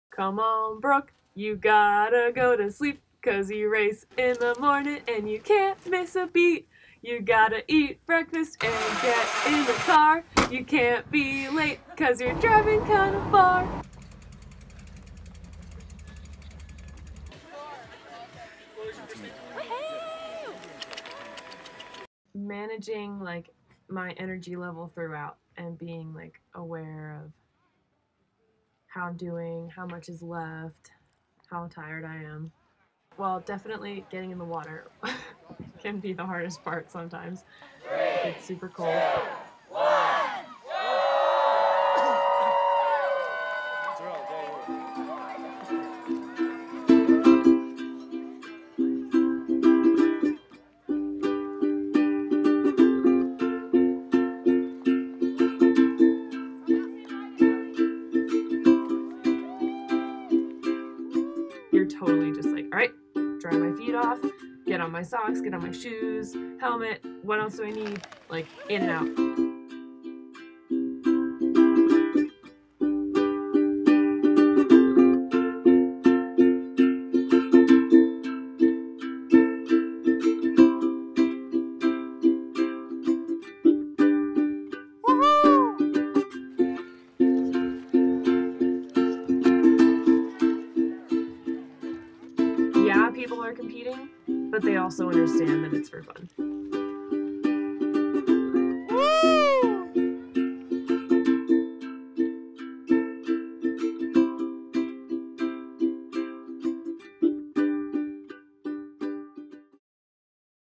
Swim Bike Run
A film about what it takes to run a triathlon. Follow Brooke Smith through the Bear Lake Brawl 2017. Featuring a lullaby performed by the star herself, and Ukulele jams by Cory. Produced by Basement Productions 2017